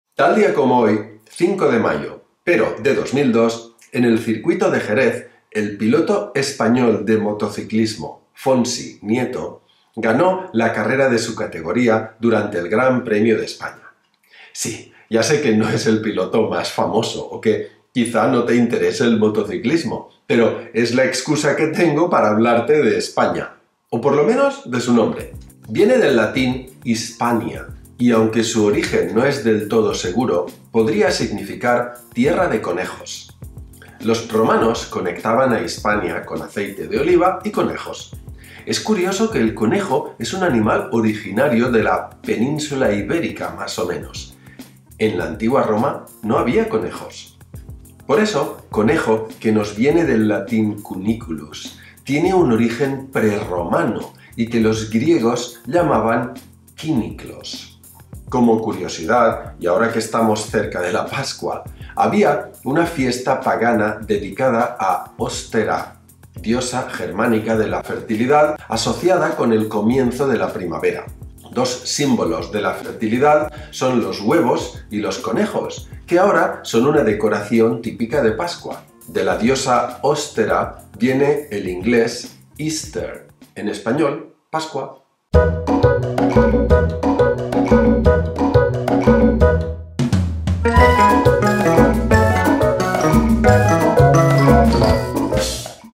Tal día como hoy, 5 de mayo. ¿De dónde viene ESPAÑA? Está conectado con la PASCUA.
Tal día como hoy, cinco de mayo pero de dos mil dos, en el circuito de Jerez, el piloto español de motociclismo Fonsi Nieto ganó la carrera de su categoría durante el Gran Premio de España. Sí, ya sé que no es el piloto más famoso, o que quizá no te interesa el motociclismo, pero es la excusa que tengo para hablarte de España, o por lo menos de su nombre. Viene del latín “Hispania”, y aunque su origen no es del todo seguro, podría significar tierra de conejos. Los romanos conectaban a Hispania con aceite de oliva y conejos. Es curioso que el conejo es un animal originario de la península ibérica más o menos, en la antigua Roma no había conejos. Por eso conejo, que nos viene del latín “cuniculus”, tiene un origen prerromano y que los griegos llamaban “kýniklos”. Como curiosidad, y ahora que estamos cerca de la Pascua, había una fiesta pagana dedicada a Ostera, diosa germánica de la fertilidad asociada con el comienzo de la primavera. Dos símbolos de la fertilidad son los huevos y los conejos, que ahora son una decoración típica de Pascua. De la diosa Ostera, viene el inglés “Easter”, en español Pascua.